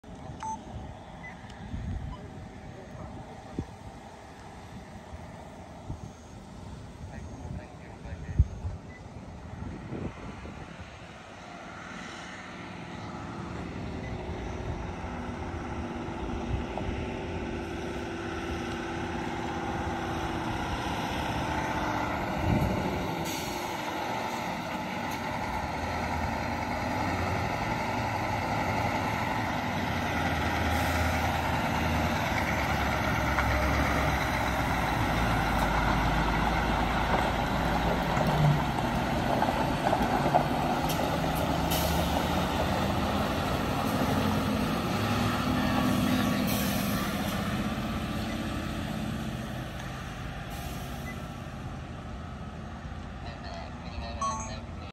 ＲＫ商会 SCANIA S650 工区内輸送
初めて見るオレンジ色とグレーの似合うスカニアは、アールケー(ＲＫ)商会さんのS650になります。
1200トンクレーンの部材を積んで、高低差のある変形Y字路を右折していきます。